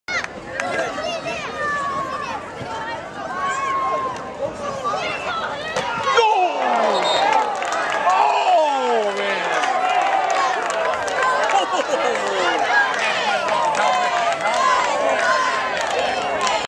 BIG Hit - Football Kickoff - Ouch Thats gotta Hurt!!!
Walled Lake Northern Football Kick-Off - Big Hit by Jackson Harris #1